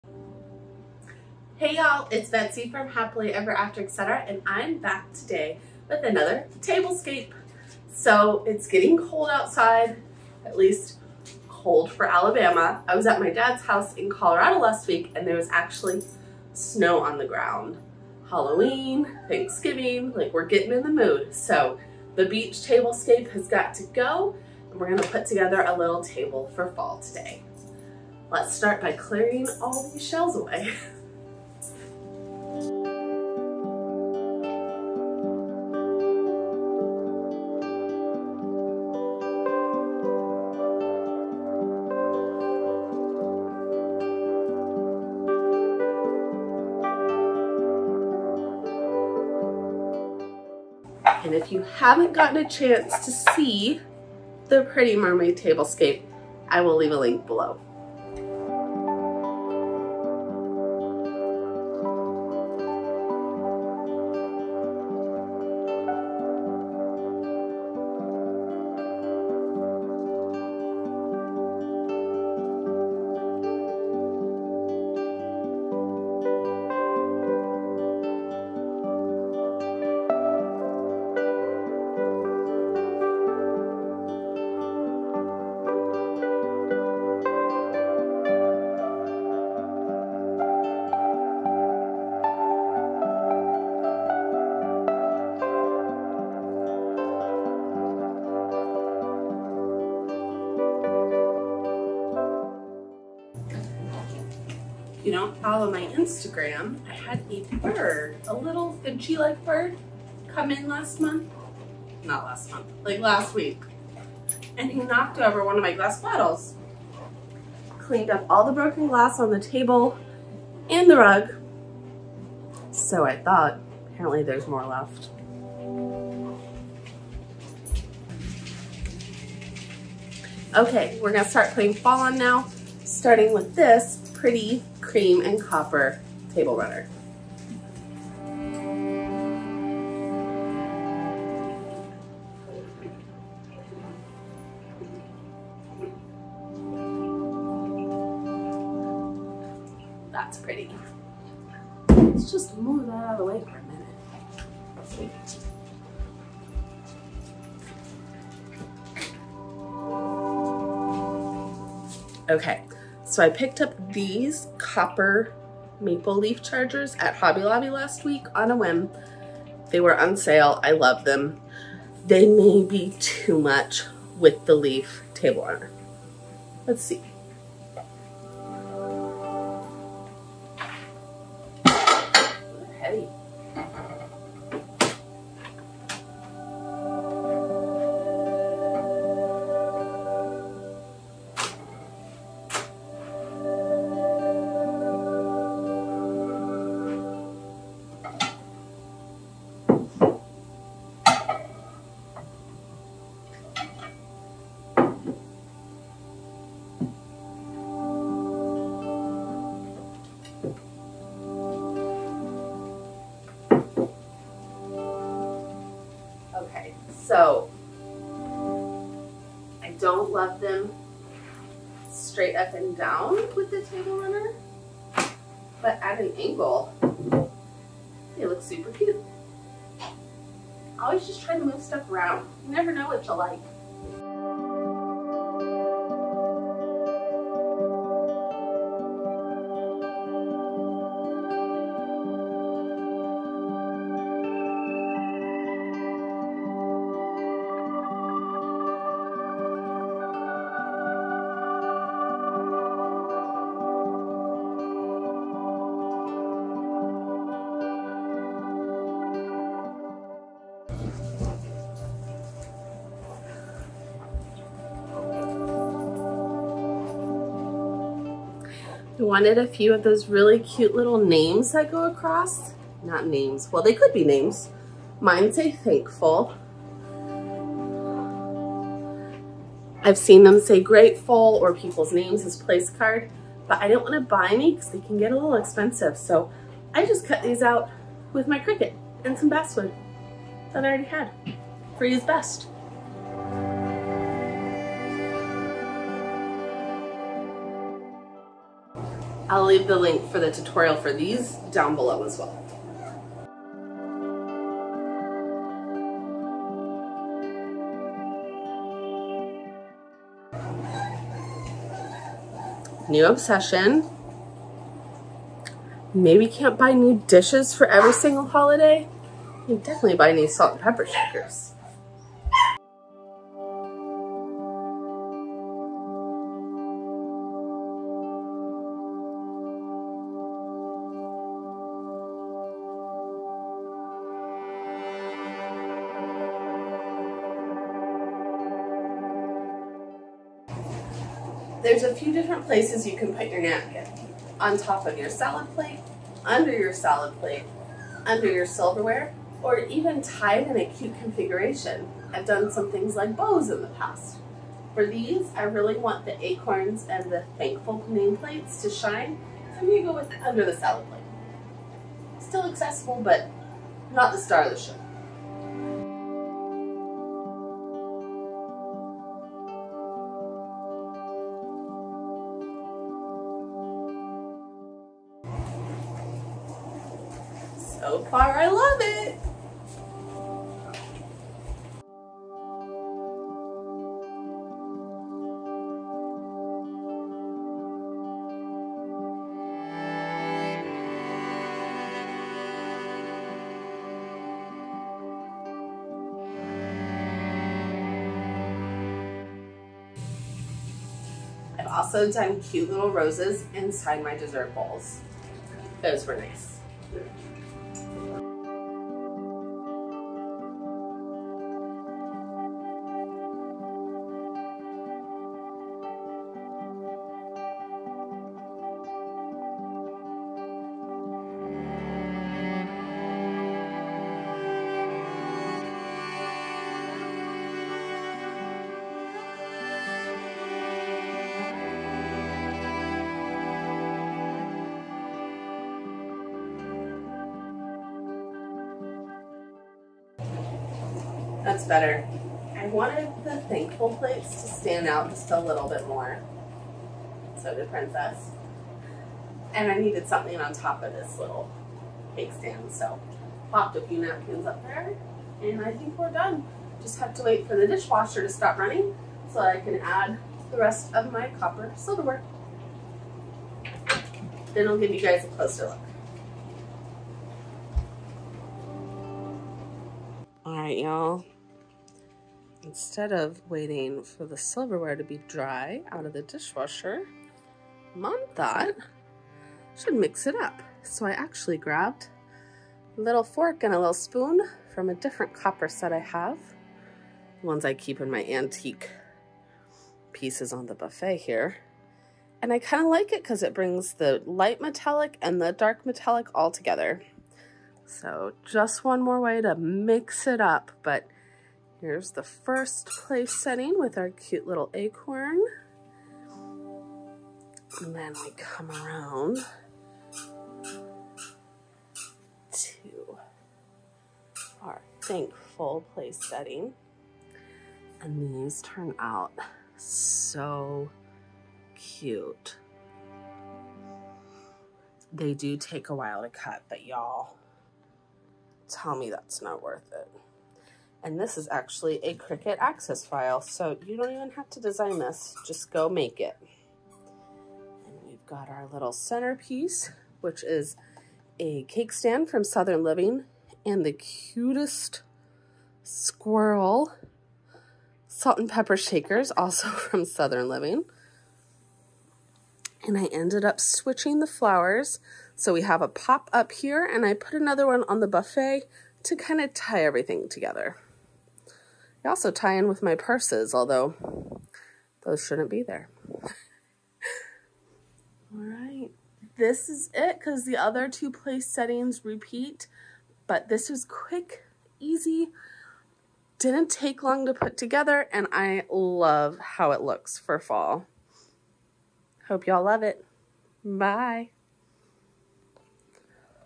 Beautiful Blush and Rose Gold Fall Tablescape
JUST GETTING STARTED?

SUPPLY LIST FOR THIS VIDEO:
*Blush Pink Dinner Plates
*Blush Pink Salad Plates
*Southern Living Cake Stand
*Squirrel Salt & Pepper Shaker
*Clear Ribbed Highball Water Glasses
*Copper Flatware